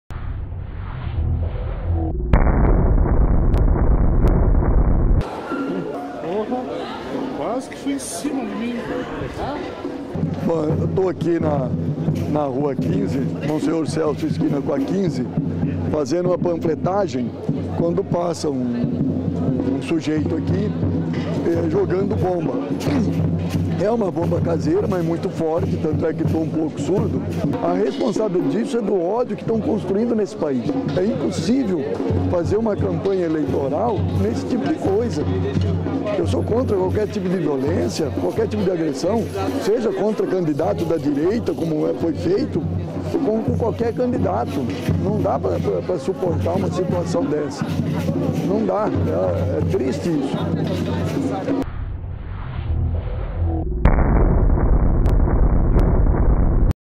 Dr Rosinha sofre atentado à bomba no Paraná
Candidato ao governo do Paraná pelo PT sofre atentado à bomba

O candidato ao Governo do Paraná, Dr. Rosinha (PT), foi alvo de um atentado a bomba, na tarde desta quinta-feira (13), quando panfletava na Rua XV de Novembro, no Centro de Curitiba.

Segundo a assessoria do petista, um desconhecido jogou o artefato no candidato na hora do rush, cheio de pessoas no local. Rosinha não foi ferido.

“O candidato e sua campanha repudiam veementemente qualquer ato de violência que fere não apenas candidatos, mas também e sobretudo a democracia. Não é aceitável que, em pleno 2018, o ódio tenha se tornado o combustível da política”, disse a campanha em nota.

Embora sendo do PT e com ideologias diferentes, Dr. Rosinha condenou recentemente o ataque ao candidato à presidência da República Jair Bolsonaro. “Dr. Rosinha sempre se colocou como candidato do diálogo e absolutamente oposto a qualquer forma de violência. Esse triste incidente apenas reforça a necessidade de lutarmos por paz e civilidade”,  disse o comunicado da assessoria.